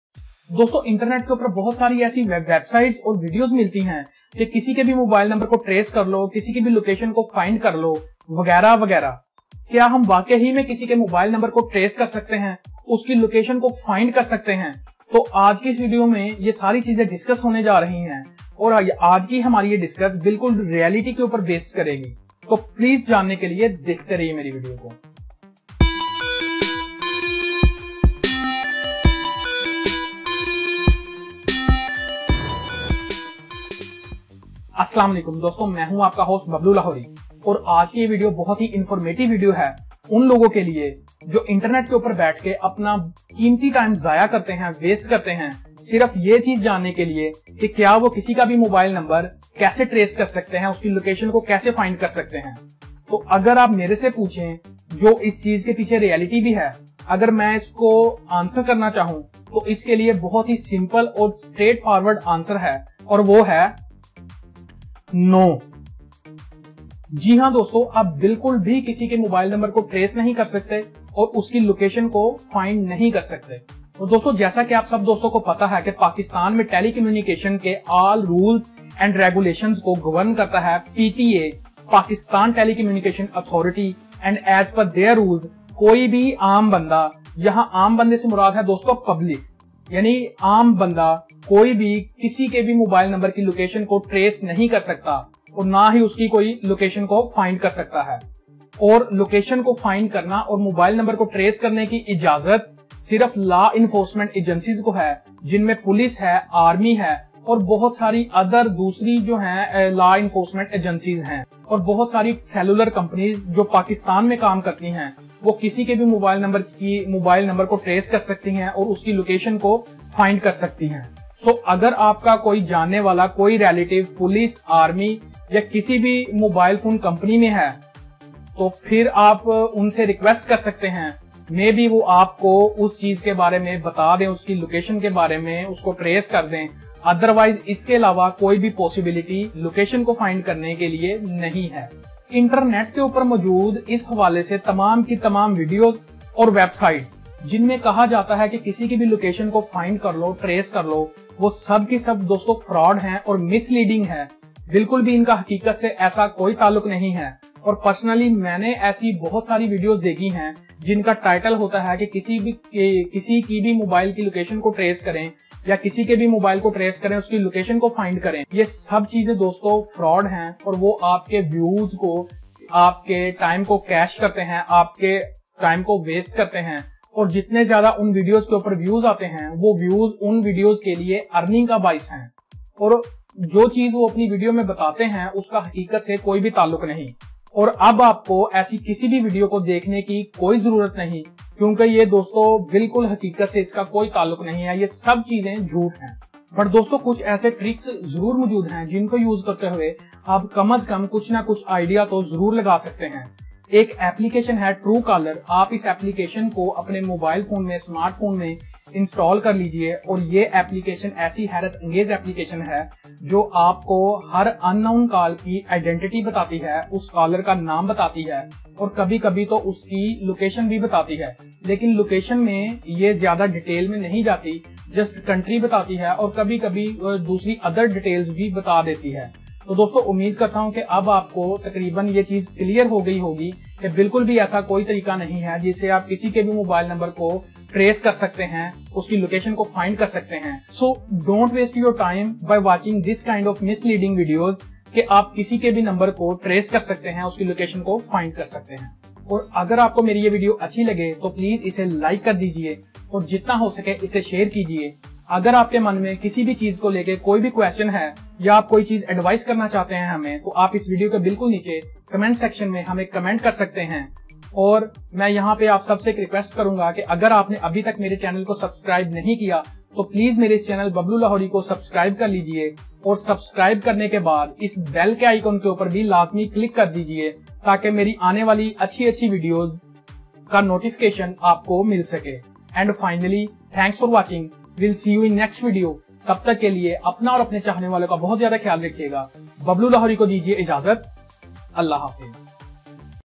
How To Track Any Mobile Number With Exact Location ? Is It True ? Reality Explained
Can we trace any mobile number and its current location from Pakistan and India ..?? Is it true ?? what is the reality behind it ??
today in this video i am going to explain all these ..
No one can find the location of any mobile phone, all the videos on  and websites having content for viewers to find the location of any mobile phone is fake and misleading..
only law enforcement  agencies and all mobile networking companies have access to find the location of any mobile phone ...other wise there is no way to public to trace mobile number and its location..So don't waste your time by watching fake videos and websites to find the locations of any mobile phone etc etc...
Stay Blessed !!